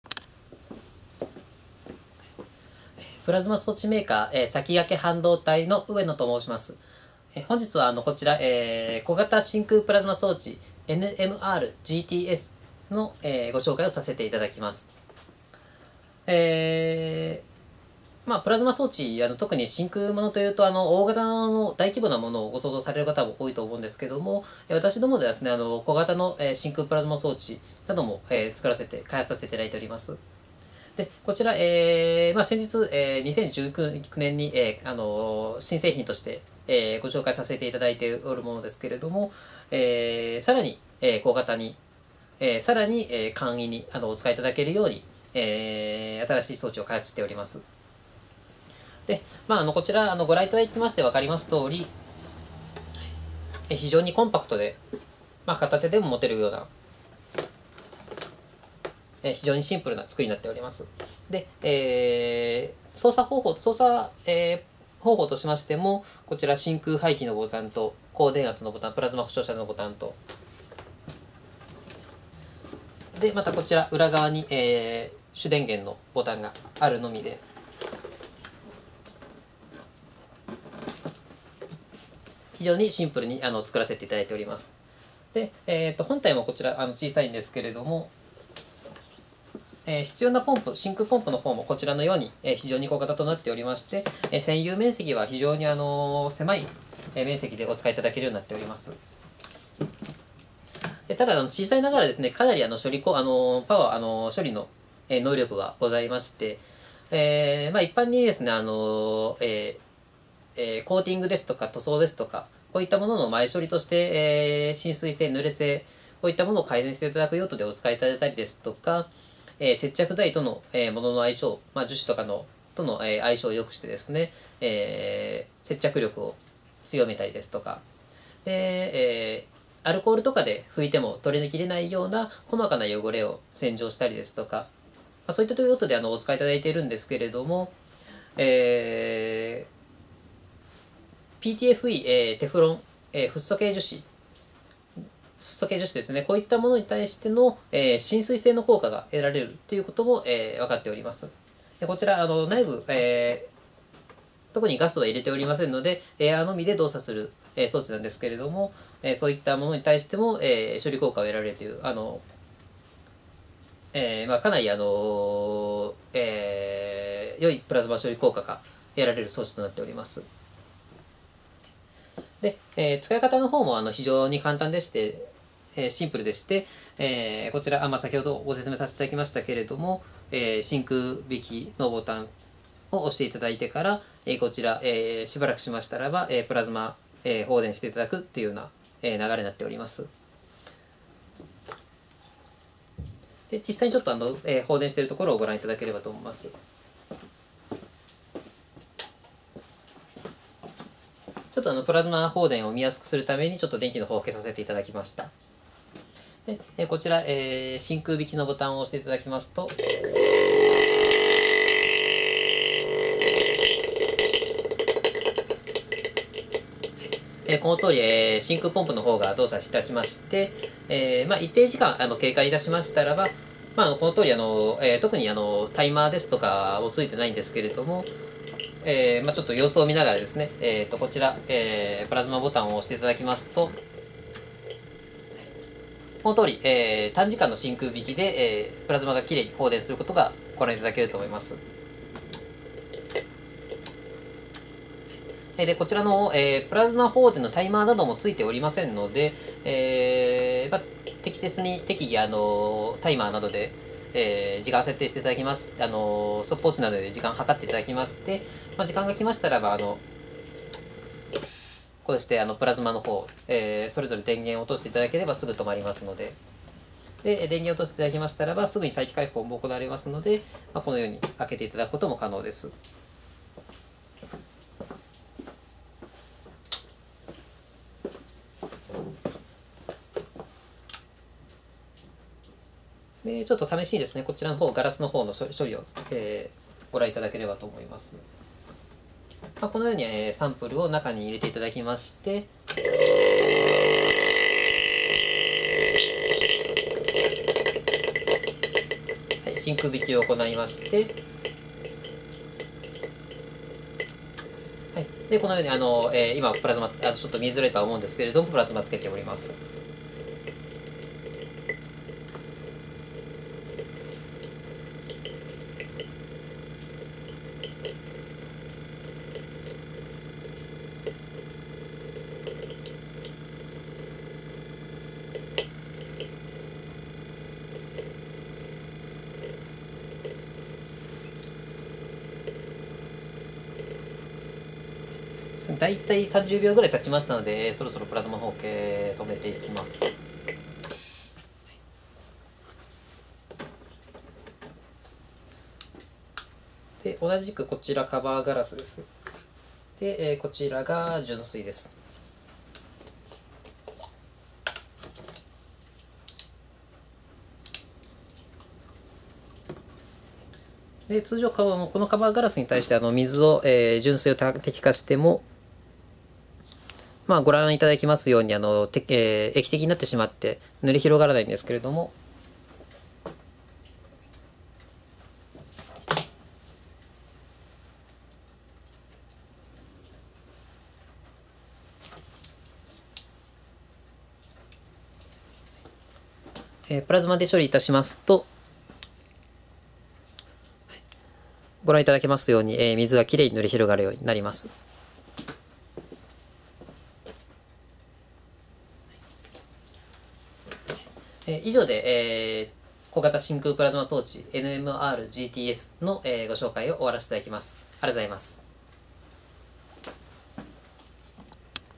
小型真空プラズマ装置 NMR-Gtsの紹介
卓上真空プラズマ装置　NMR-Gtsの製品説明です。
小型で容易にお使い頂けるのが分かります。
※魁半導体の製品です。
説明者は営業部所属の植野です。

↑こちらです。

↑こちらです。